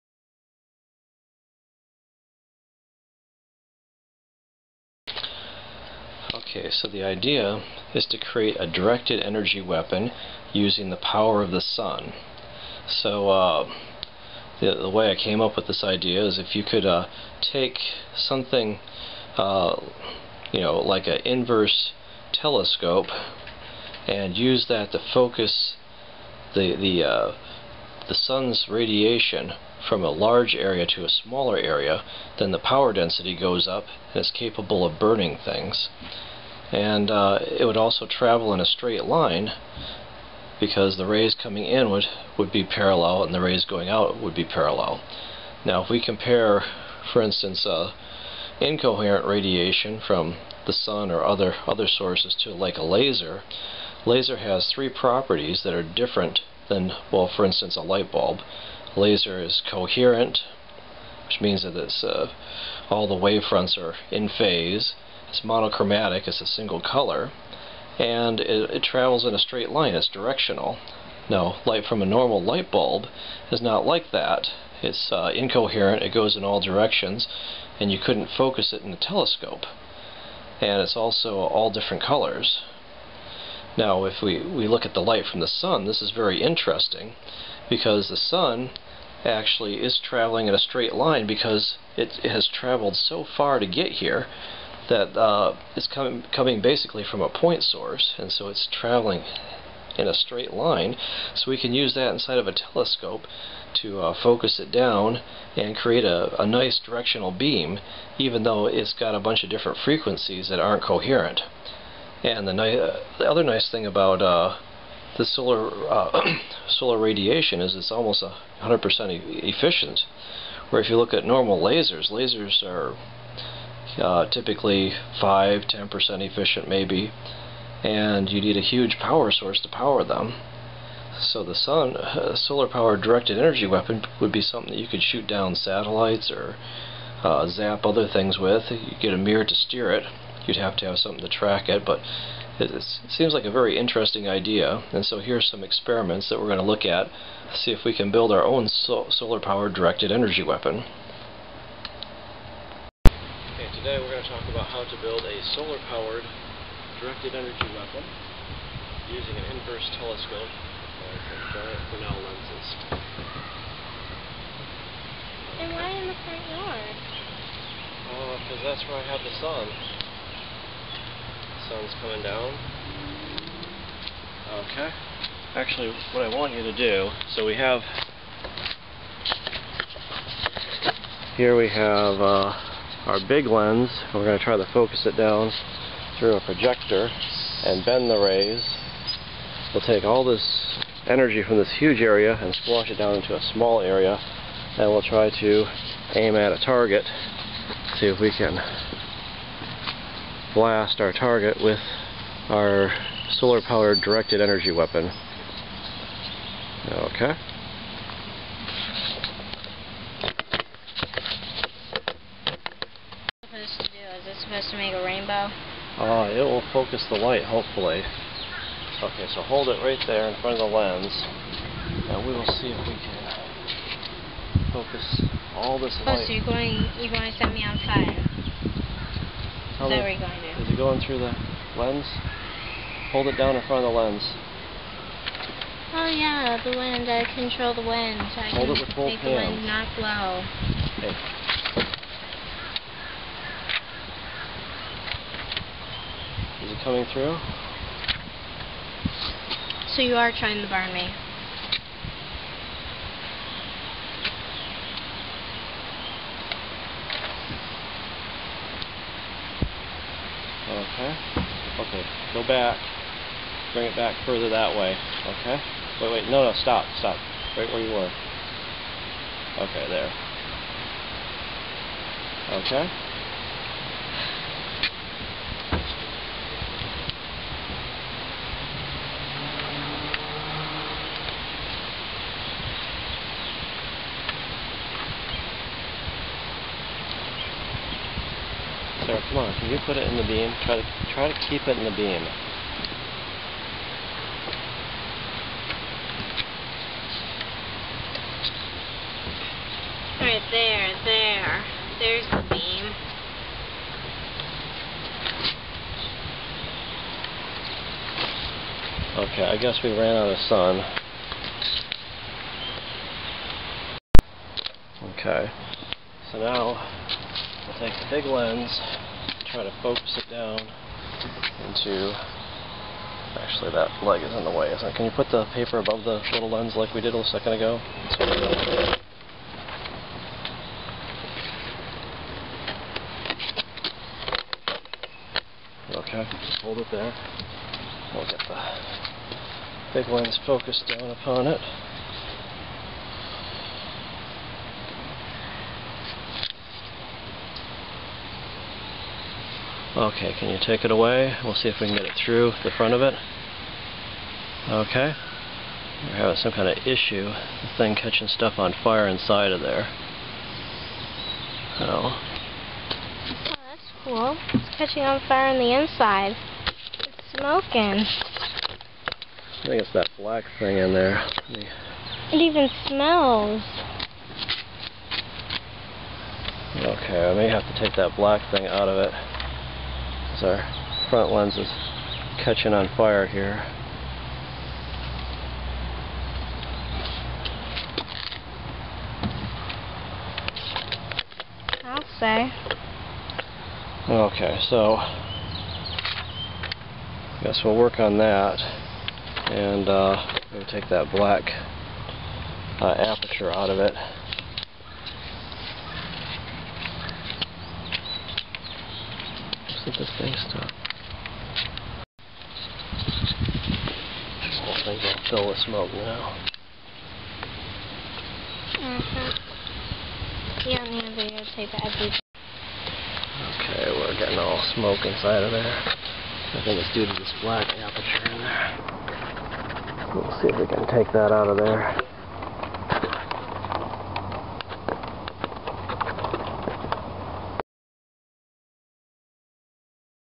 Solar Powered Directed Energy Weapon - part 1
Here is the first installment in a series of experiments designed to look at the possibility of harnessing the power of the sun using telescope like optics to create a high power burning laser like beam directed energy weapon with incoherent radiation to burn things at a distance.